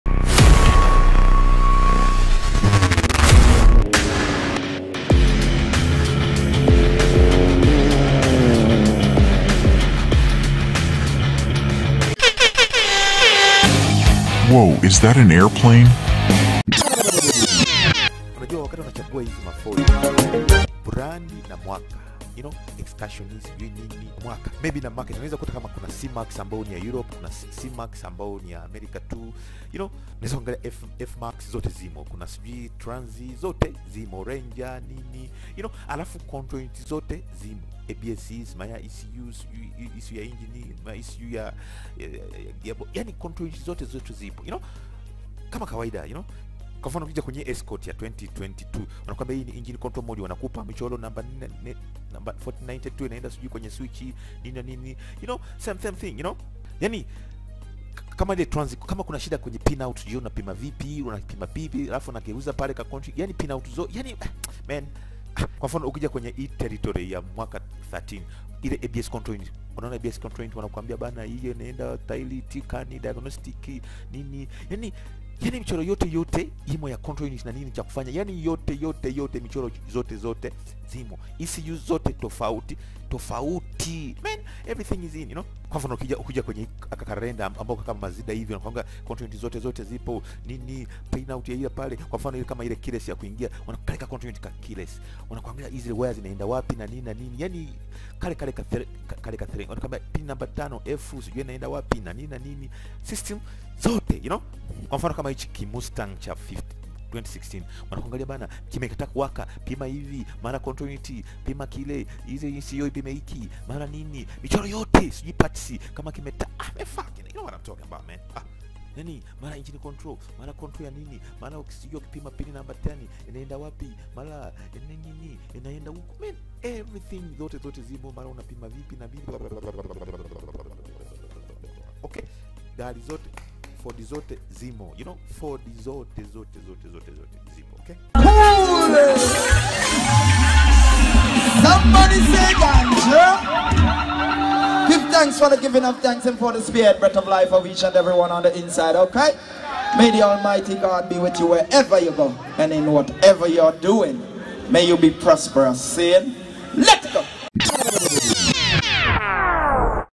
FORD ECU database with pinouts (terminals) ! Select🧐By Model, year, or, model code number🤷‍♂️!
We compile ECU PINOUTS for every all FORD's!
As demonstrated in the video, you may select by specific Engine Configuration, AND also, which is far better, choose by specific model AND year, as demonstrated in the👆👆 video!